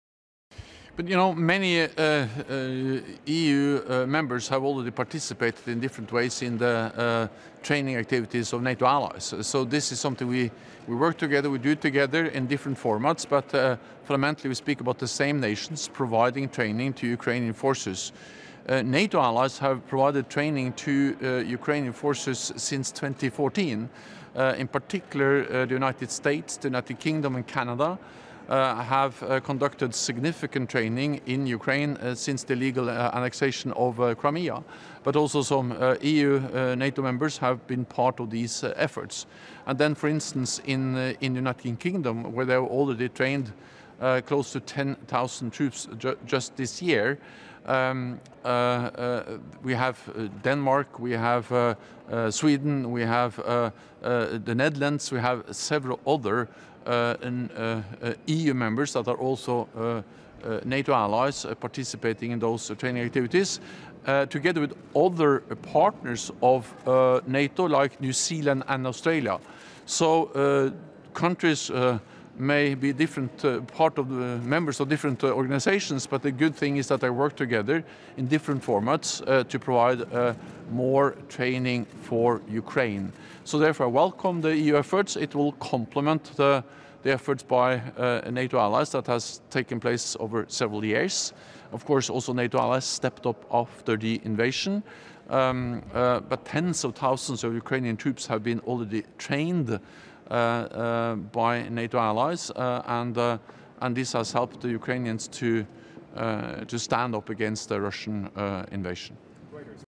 EU members have already participated in different ways in the training activities with NATO Allies!
You know, many EU members have already participated in different ways in the training activities with NATO Allies. So this is something we work together. We do it together in different formats, but primarily we speak about the same nations providing training to Ukrainian forces.

NATO Allies have provided training to Ukrainian forces since 2014. In particular, the United States, the United Kingdom and Canada, have conducted significant training in Ukraine since the illegal annexation of Crimea, but also some EU NATO members have been part of these efforts. And then for instance, in the United Kingdom, where they're already trained close to 10,000 troops just this year.

We have Denmark, we have Sweden, we have the Netherlands, we have several other EU members that are also NATO Allies participating in those training activities. Together with other partners of NATO like New Zealand and Australia. So countries may be members of different organizations but the good thing is that they work together in different formats to provide more training for Ukraine. So therefore welcome the EU efforts, it will complement the efforts by NATO Allies that has taken place over several years.

Of course, also NATO Allies stepped up after the invasion. But tens of thousands of Ukrainian troops have been already trained by NATO Allies and this has helped the Ukrainians to just stand up against the Russian invasion.

eudebates the unique initiative aiming to promote debate, dialogue, knowledge, participation and communication among citizens.